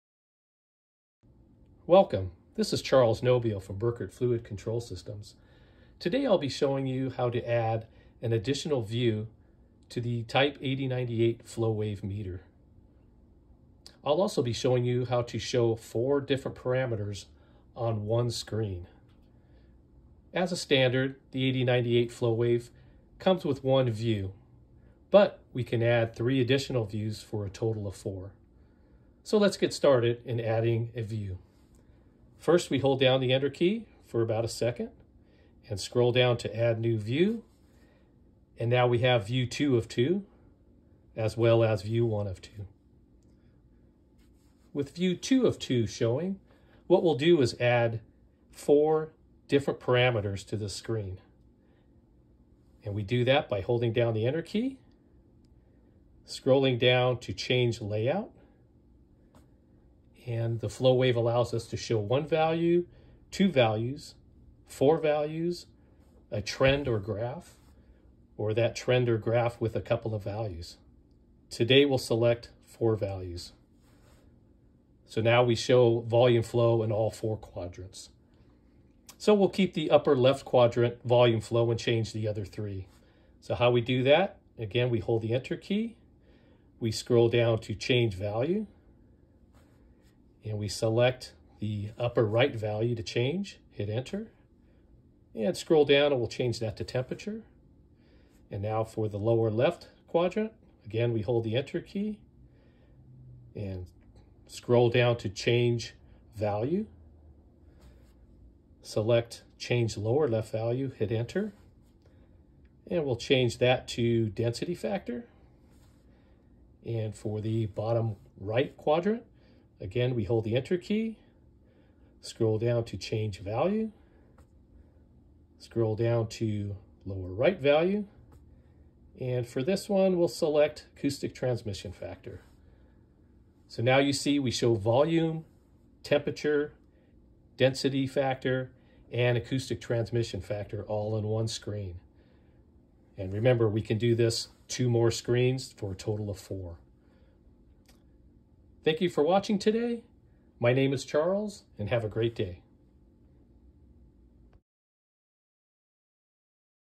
Burkert FLOWave Type 8098 - adding views parameters to display screen
To add view parameters to the display screen of the Type 8098 FLOWave, follow these simple instructions.